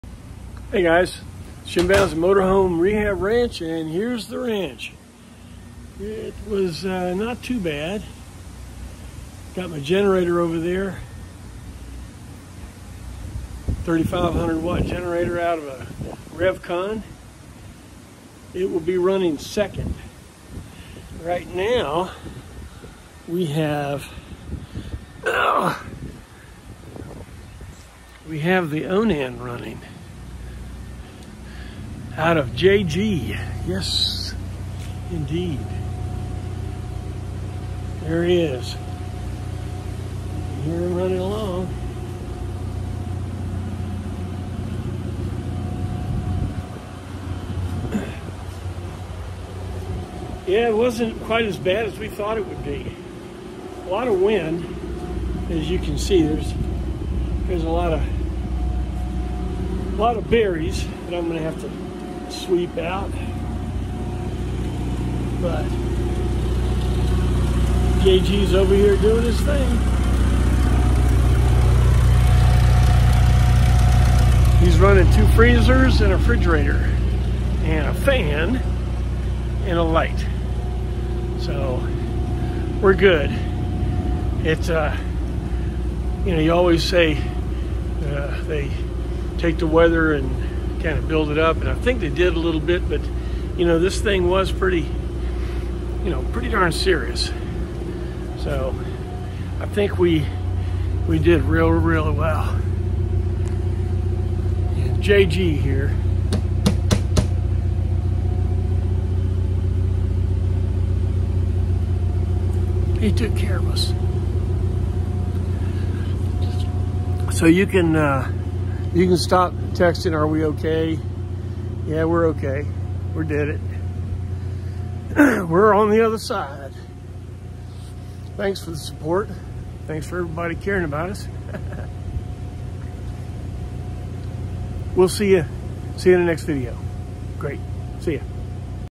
Hurricane Milton is over, we survived
Thanks for the inquiries….. we made it to the other side….. alote of limbs down, power IS out but JayGee is pomping out power to run 2 freezers, a frig and fan.  4 on, 2 off then the 3500 watt Kohler will take over the next 4.

Thanks for everyone’s concern…. We’re good.

Hey and to those at the Cali GMCMI convention……. Wish I could be there but you understand.  Enjoy the rally, I hope to make the next one in March….. that’s not during hurricane season!